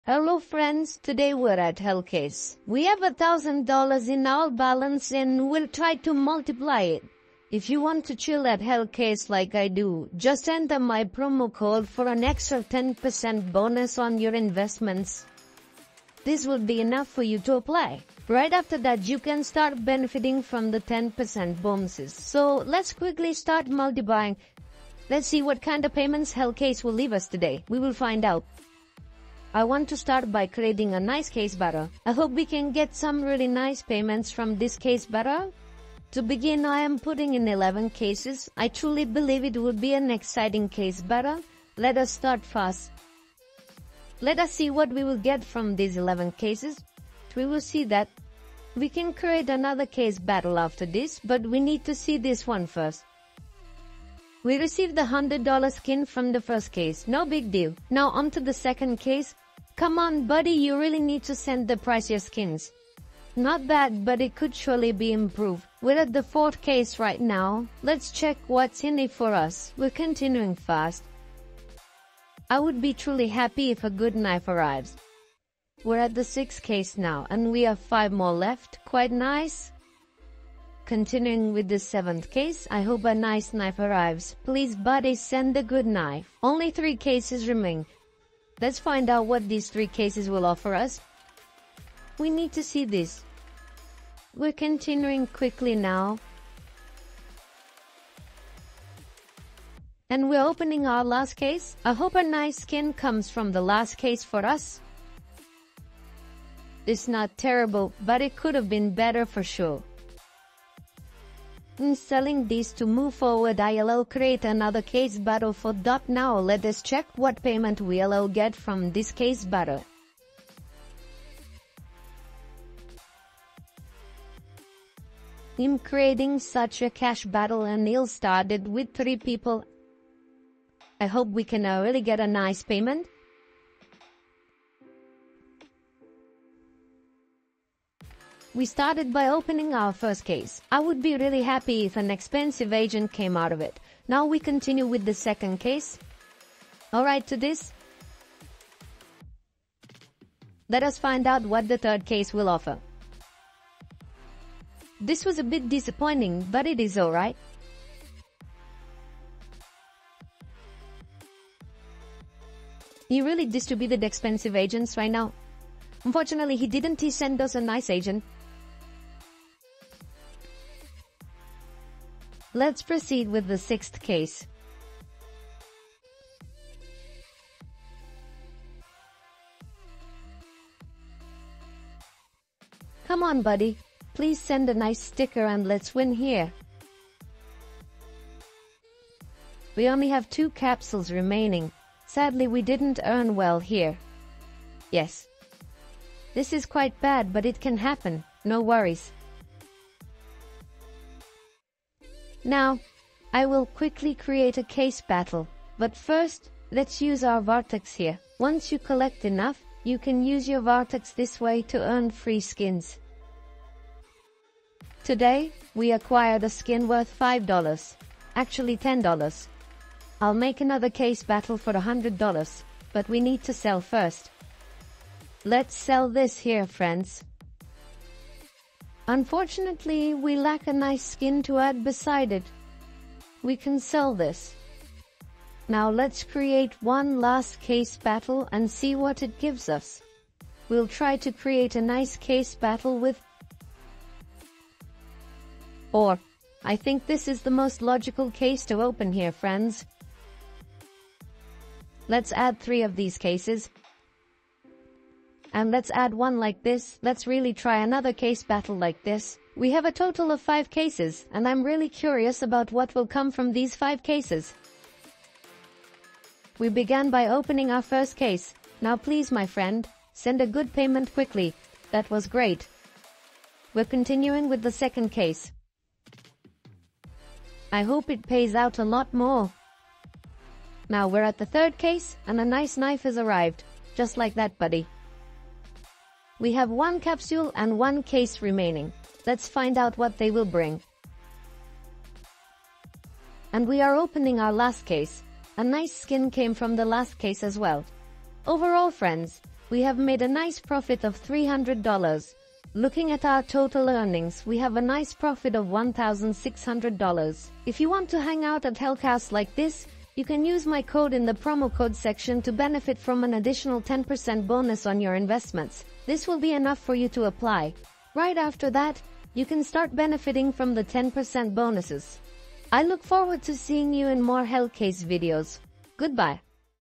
HELLCASE I WON MOST EXPENSIVE M4A4 HOWL (FACTORY NEW) IN THE BATTLE! - hellcase
🔥For %10 Deposit Code

Hello, I'm Sanyy. Today, we tried to win great skins by doing Hellcase Case Opening, and Hellcase Case Battle, in Hellcase.

Skype: live:.cid.e8bb226dc9138a95

🔥 My other case opening videos 🔥